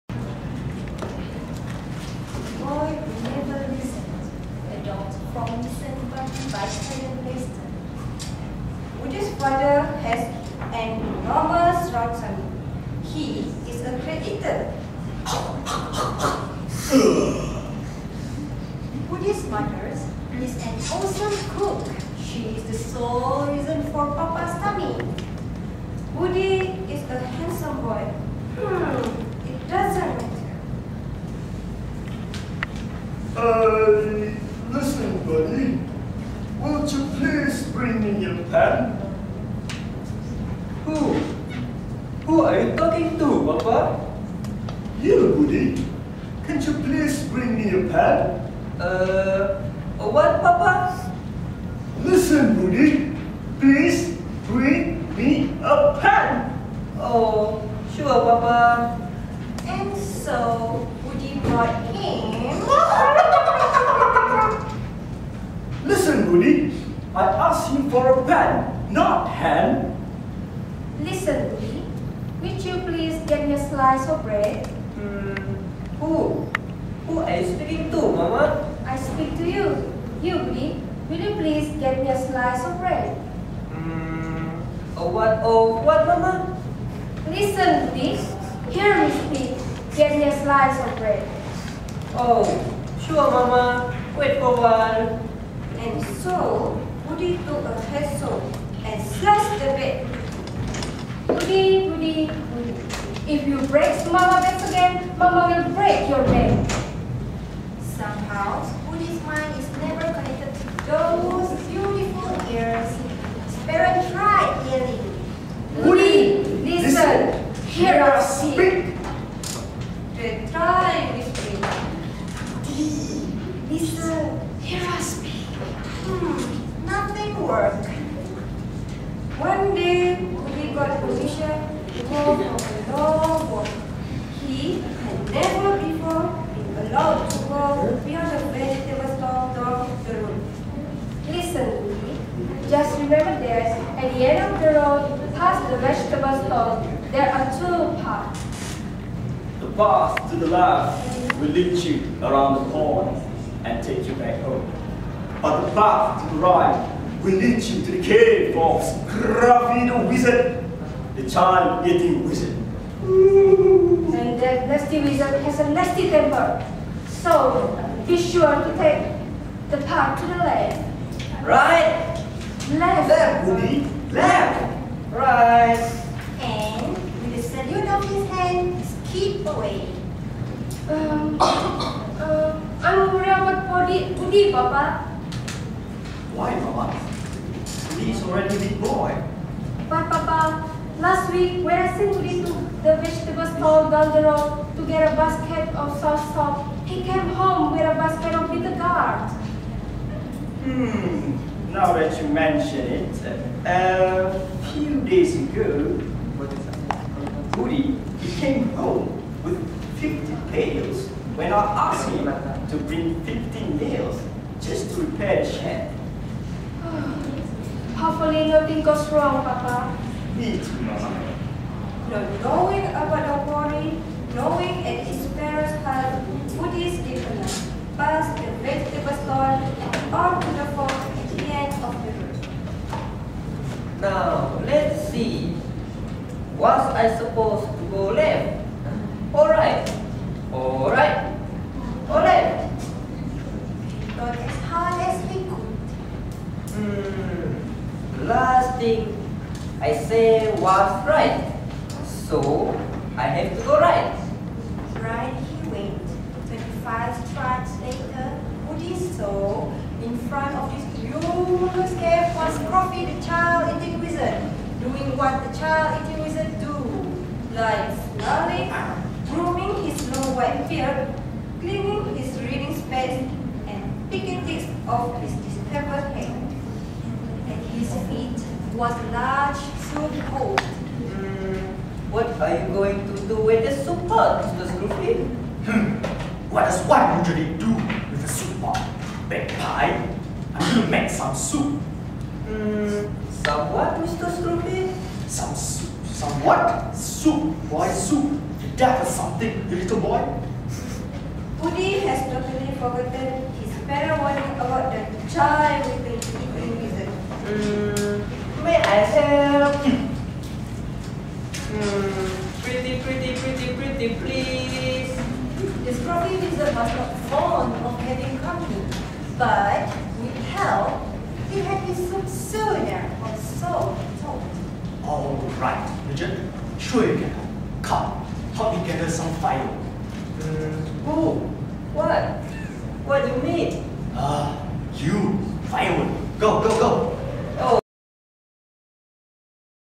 ESE Readers' Theatre Highlights: The Boy Who Never Listened.
ESE Reader's Theatre (Sem 2, 2013/2014). Short highlights of the readers' theatre (Unedited).